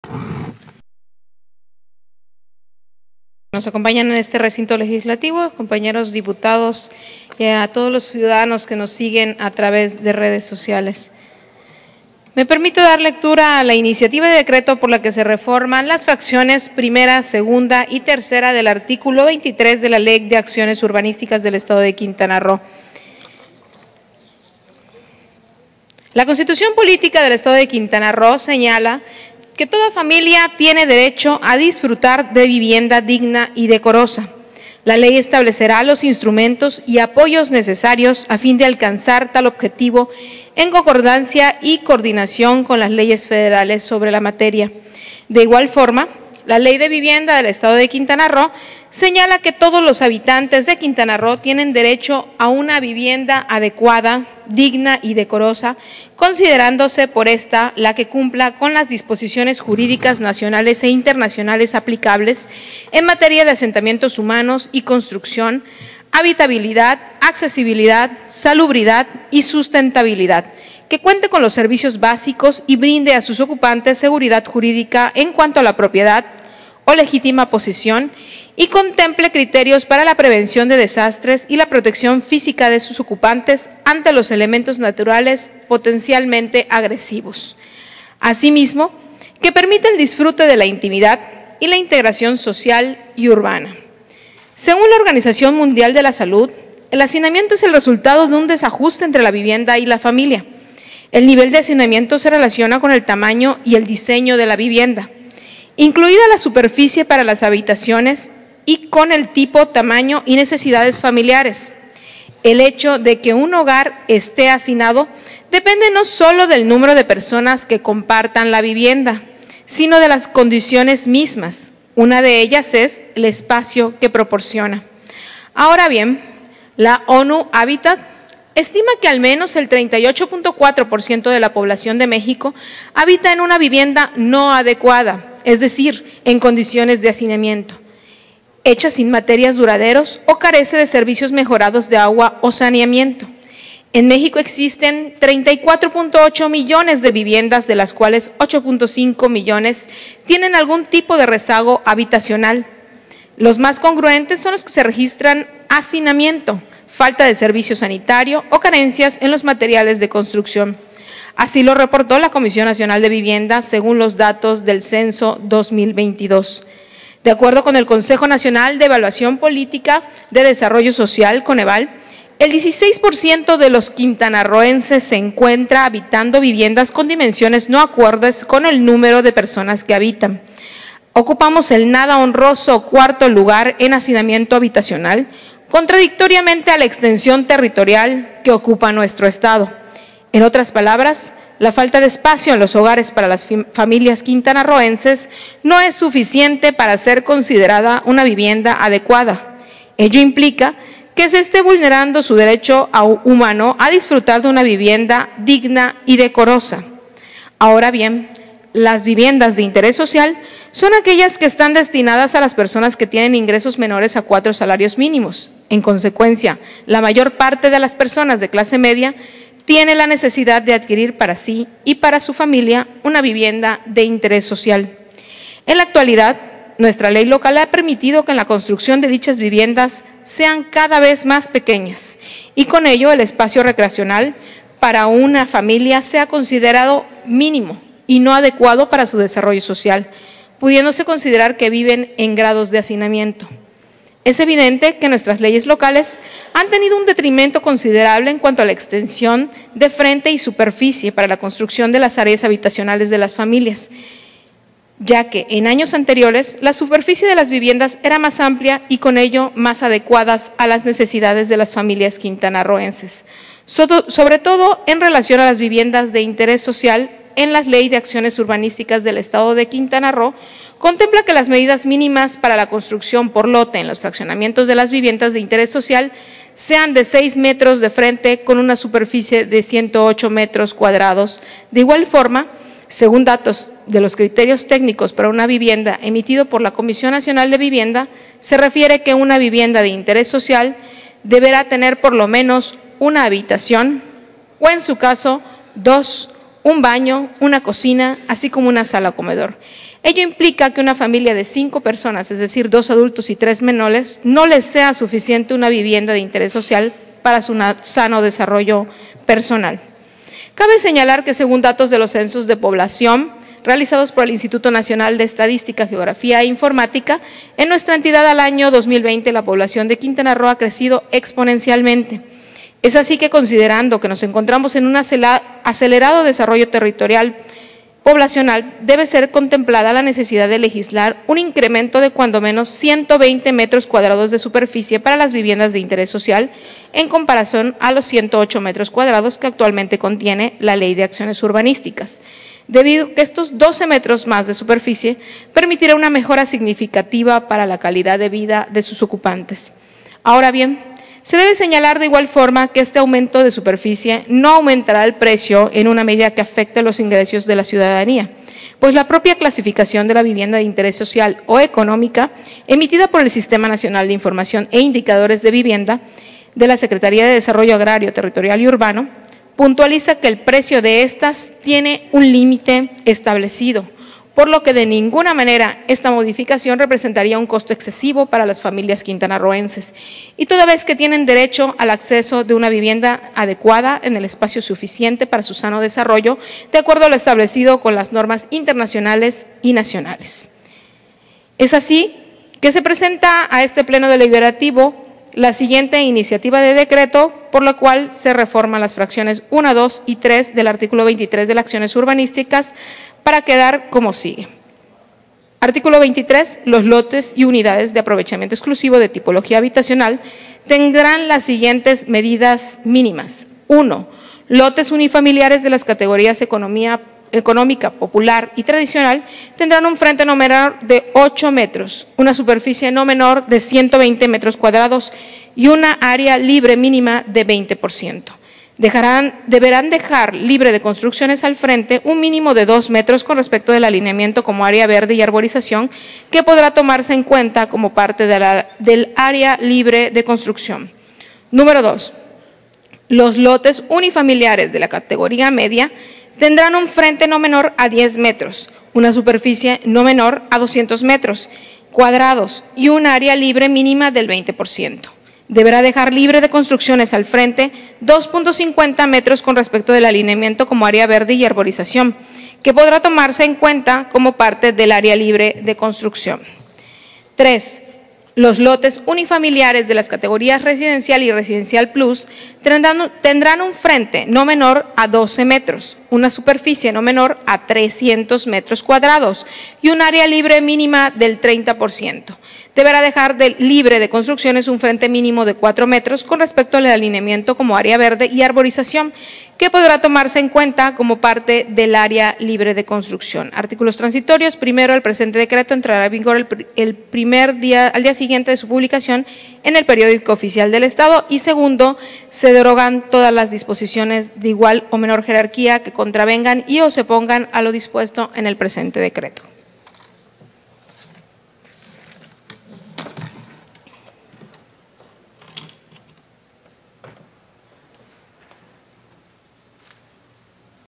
Intervención Dip. Cinthya Yamilie Millán Estrella. PAN (06/Diciembre/2023)
Intervención de la Dip. Cinthya Yamilie Millán Estrella, del PAN, para la lectura de la Iniciativa de Decreto por la que se reforman las fracciones I, II y III del artículo 23 de la Ley de Acciones Urbanísticas del Estado de Quintana Roo; que presenta en su claidad de Presidenta de la Comisión de Defensa de los Límites de Quintana Roo y Asuntos Fronterizos de la H. XVII Legislatura del Estado. Publicada en la Gaceta Parlamentaria Año II número 25 Ordinario de fecha 05 de diciembre de 2023